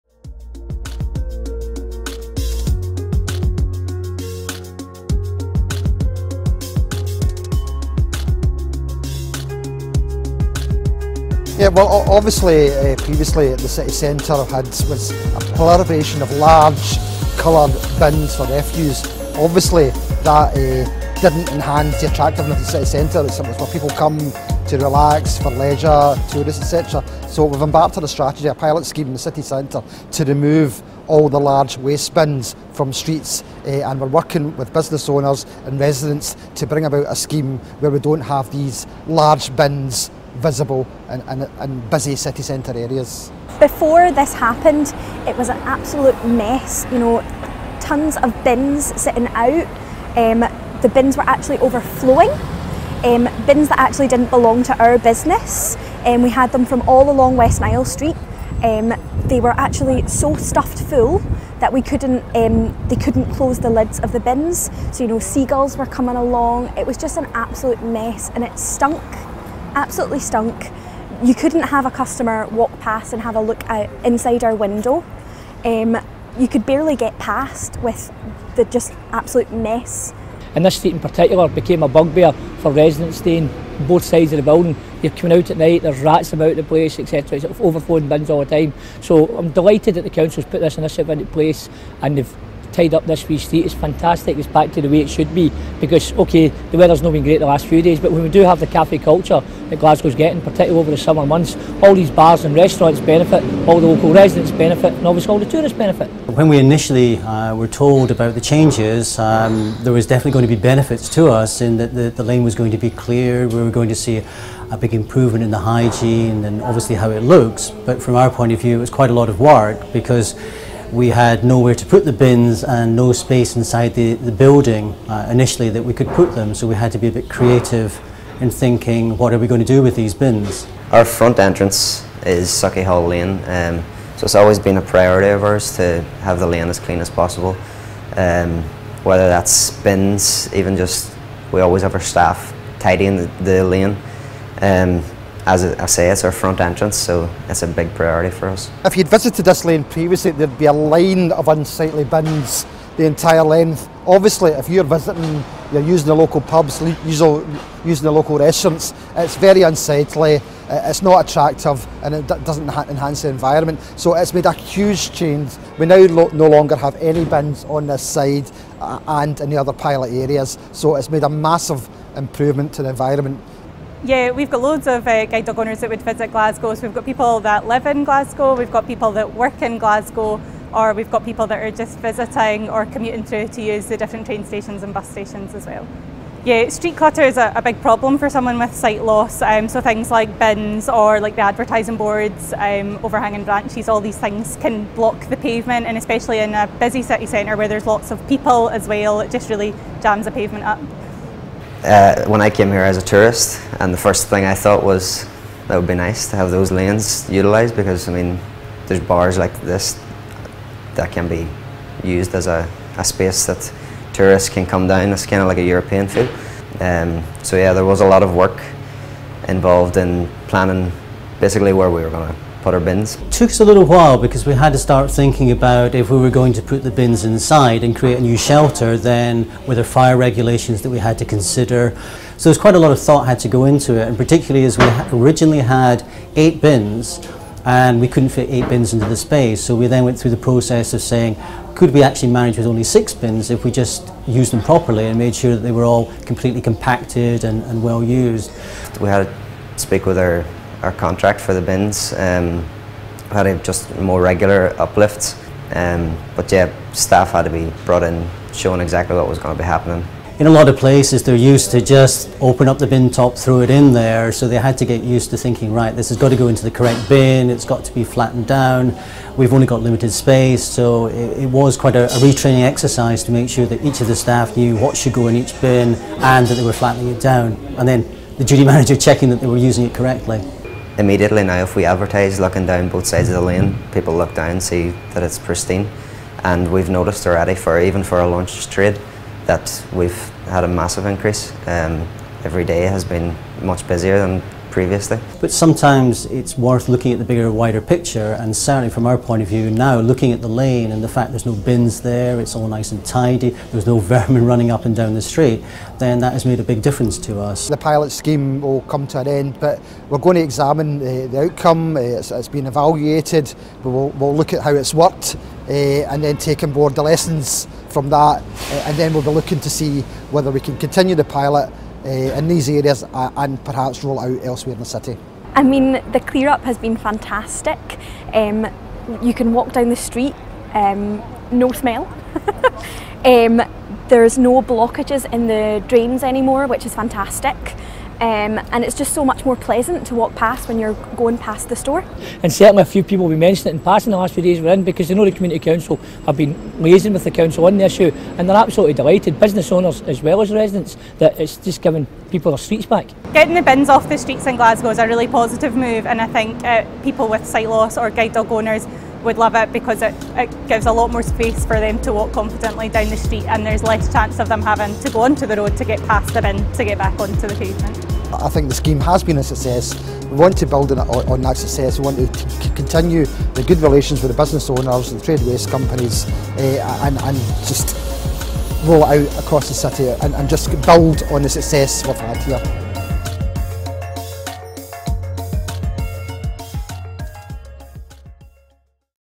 Commercial Waste
A film I made in 2017 about the problem of commercial waste and how to resolve it.